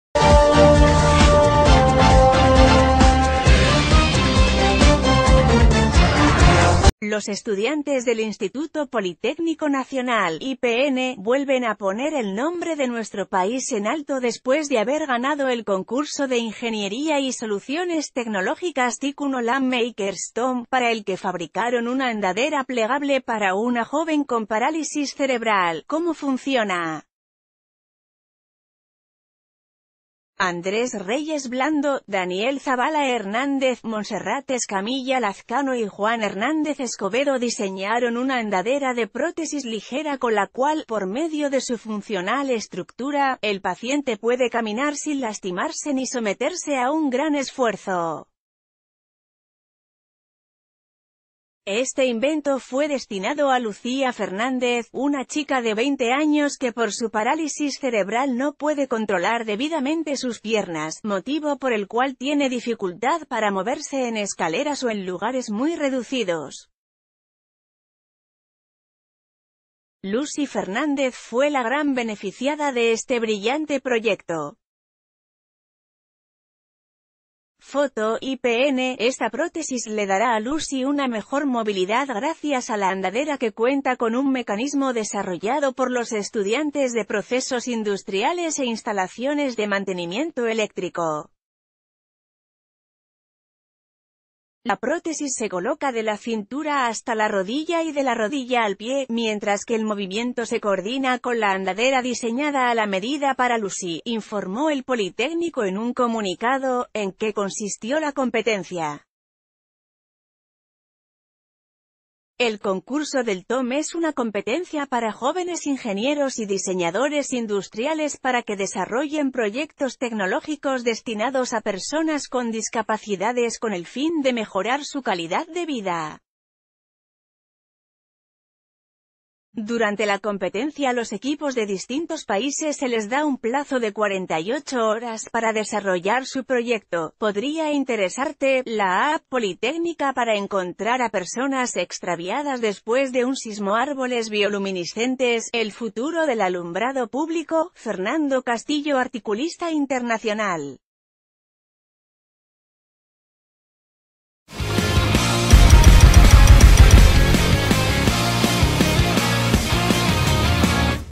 Estudiantes del Poli ganan concurso de tecnología para personas con discapacidades
Los estudiantes del Instituto Politécnico Nacional (IPN) vuelven a poner el nombre de nuestro país en alto después de haber ganado el concurso de ingeniería y soluciones tecnológicas Tikkun Olam Makers (TOM), para el que fabricaron una andadera plegable para una joven con parálisis cerebral.¿Cómo fu...